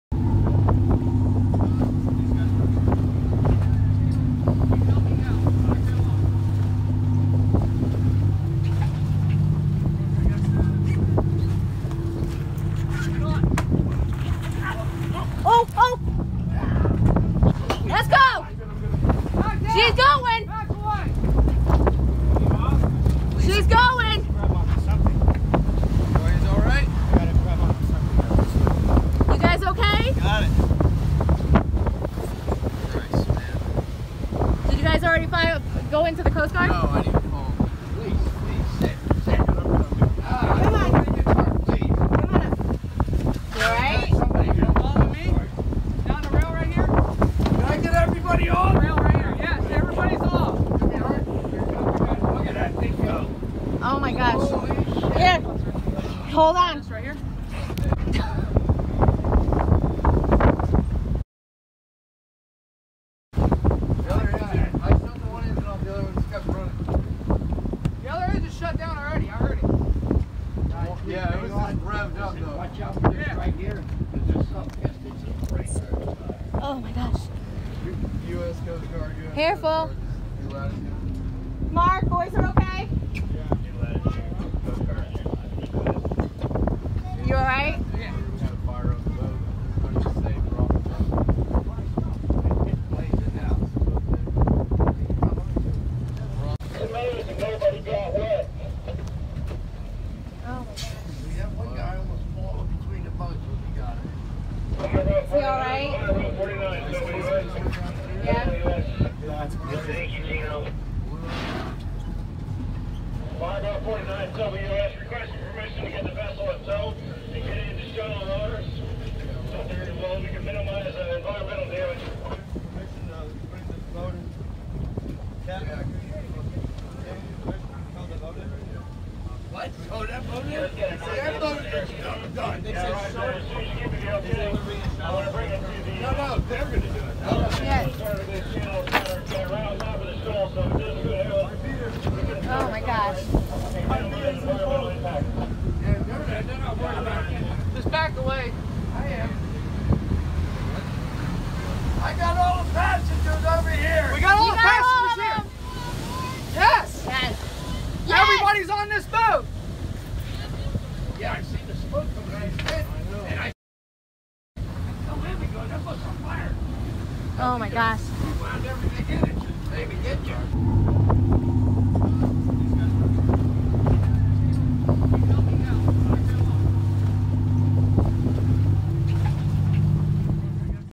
Charter boat catches fire in MINUTES off Florida coast
This group of friends were lucky when another boat was nearby, as theirs caught fire off Fort Lauderdale Beach, Florida. They all jumped onto the other vessel and watched it being engulfed by flames.